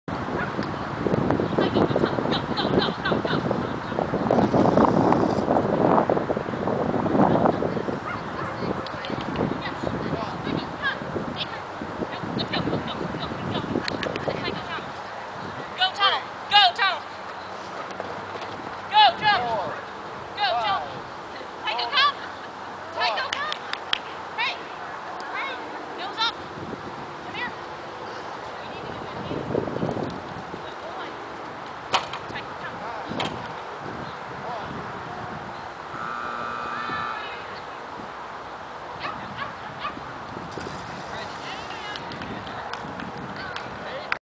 Tycho - 10/3/10 Novice FAST (NQ)
I had I plan before heading to the start line, but it dissolved somewhat once Tycho and I actually started running.  The a-frame went as planned; I signaled for it but just kept running since I knew Tycho was likely to skip it.  The only problem was that we were just a bit slower than I expected, so we missed the Q due to time faults.  :(  I was happy with Tycho's attitude, though, since he never abandoned me.

Score: 48
Time: 39.64